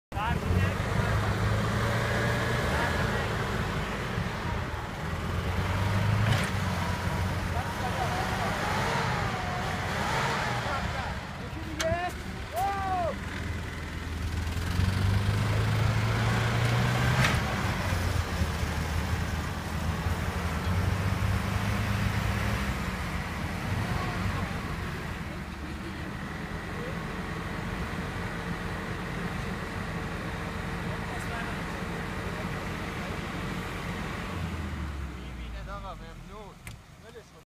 Zanjan Offroad
zanjan offroad with our cars today 1392/10/12 (2014/01/01)
Toyota fj-60 series And Jeep Km-410